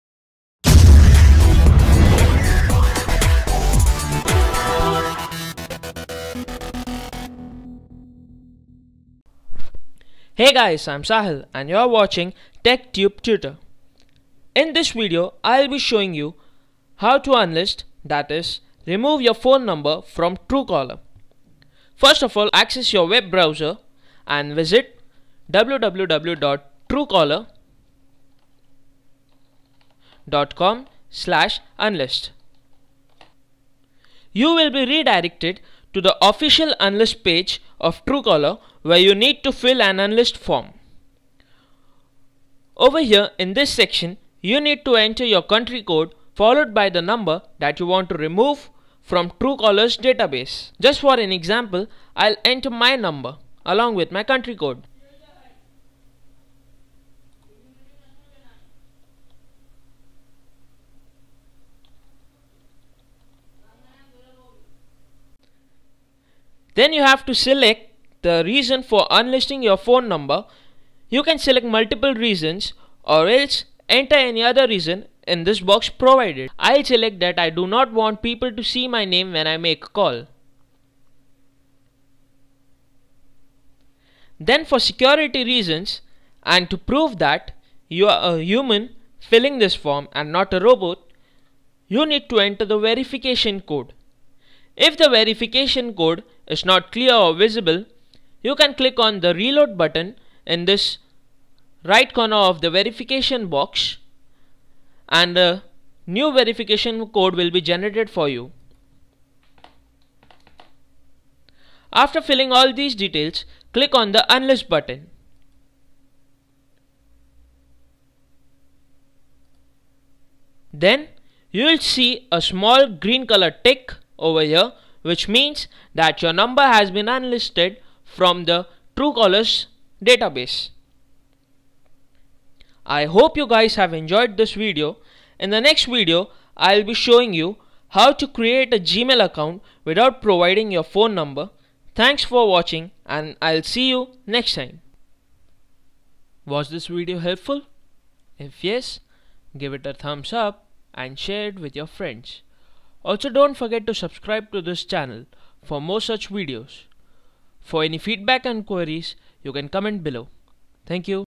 How To Unlist (remove) a phone number from truecaller?
Learn How To Unlist (remove) your phone number from True Caller.

You will be redirected to the official unlist page of truecaller where you need to fill an unlist form.

In that form, you have to enter the country code followed by the phone number that you want to remove from truecaller's database.

Then you have to select the reason for unlisting your phone number, you can select multiple reasons or else enter any other reason in the box provided. Then for security reasons you need to enter the verification code.

After filling all these details click on unlist button, and your number will be removed from truecaller's database.